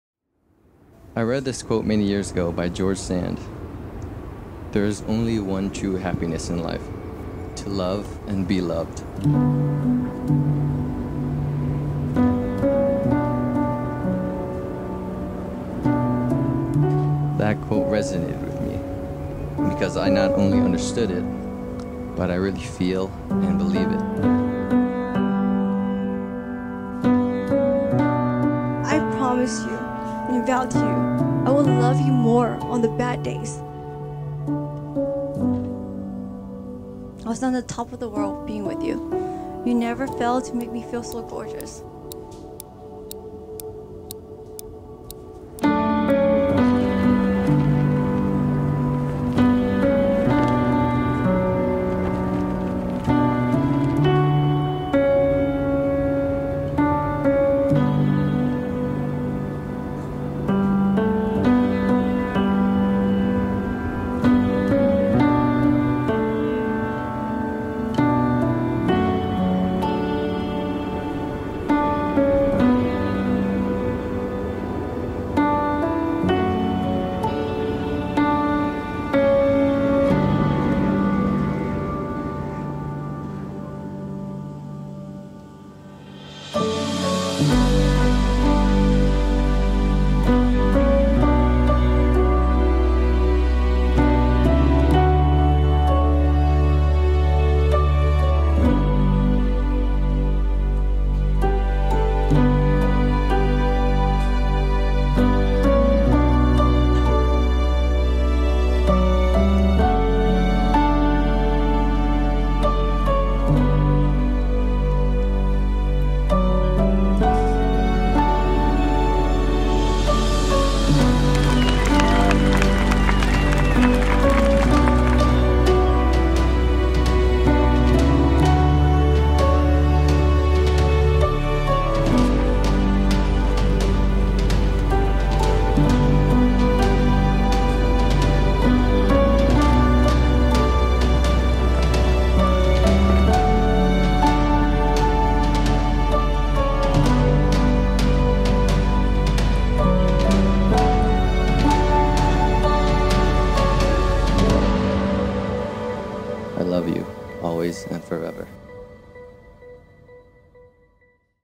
05.26.2019 - Kieu and Phuong - Trailer
It was a honor for us to capture the wedding of Kieu Nguyen and Phuong Phan. The day was magical.
Wedding held at Hanford Ranch Winery & Events in Galt California.